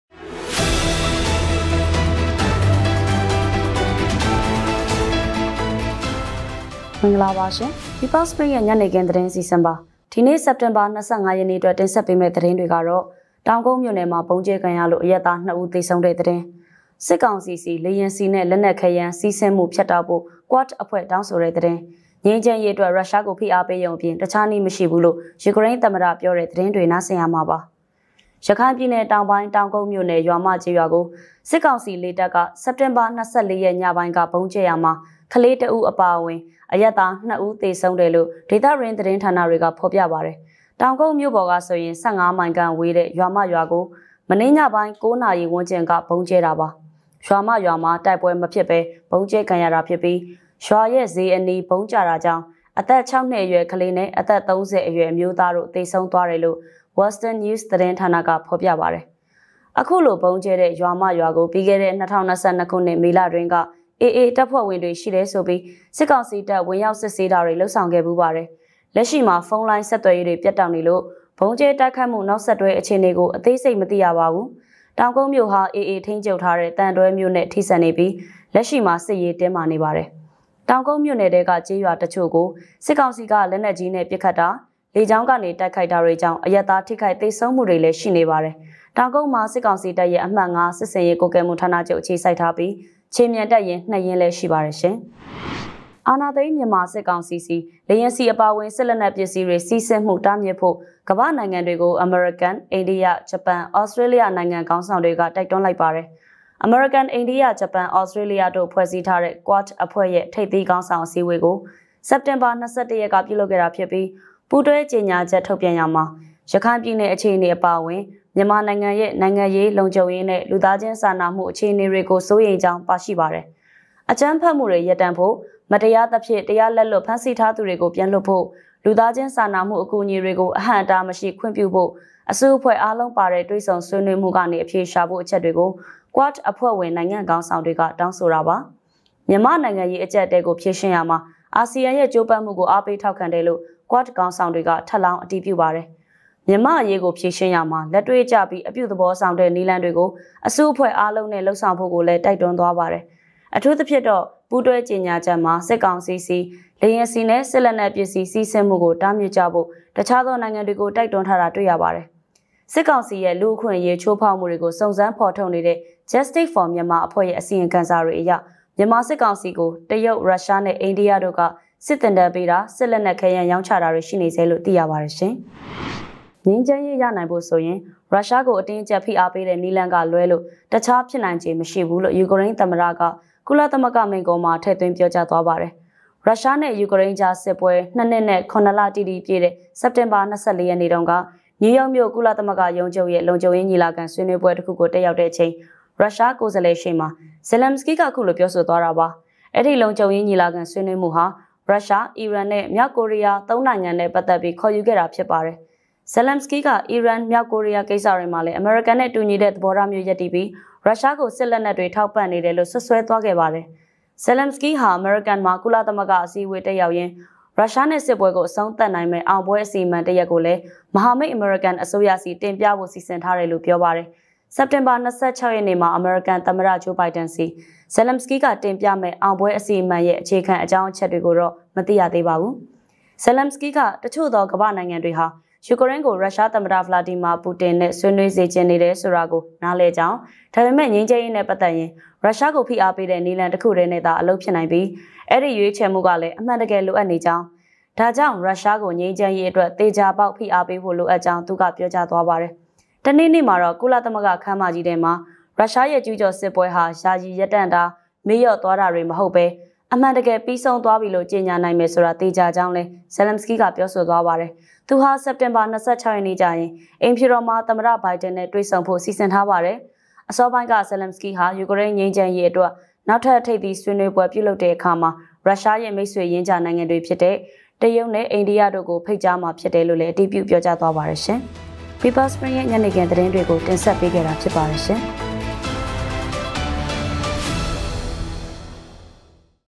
ညနေခင်းသတင်း / People's Spring
"တောင်ကုတ်မြို့နယ်မှာ ဗုံးကြဲခံရလို့ အရပ်သား ၂ ဦး သေဆုံးမှု၊ စစ်ကောင်စီဆီ လေယာဉ်ဆီနဲ့ လက်နက်ခဲယမ်း စီးဆင်းမှု ဖြတ်တောက်ဖို့ Quad အဖွဲ့တောင်းဆို၊ ငြိမ်းချမ်းရေးအတွက် ရုရှားကို  ဖိအားပေးရုံအပြင် တခြားနည်း မရှိဘူးလို့ ယူကရိန်း သမ္မတပြော"

ဒီနေ့စက်တင်ဘာလ ၂၅ ရက်မှာဖြစ်ပွားခဲ့တဲ့ သတင်းတွေထဲကအချို့ကို ကောက်နုတ်တင်ဆက်ပေးလိုက်ပါတယ်။